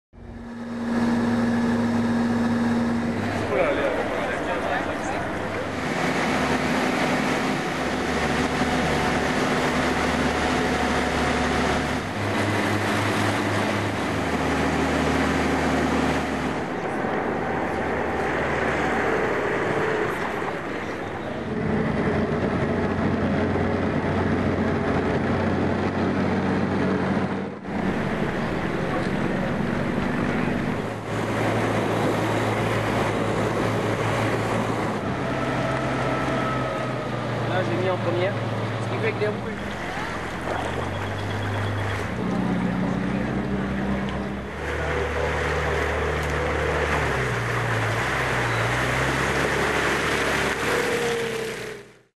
Amphibious car show gets underway in...
The 26th meeting of amphibian cars is taking place at the moment in Saint-Blaise, canton of Neuchatel. It is the largest event of this kind in Europe, 55 cars are on site.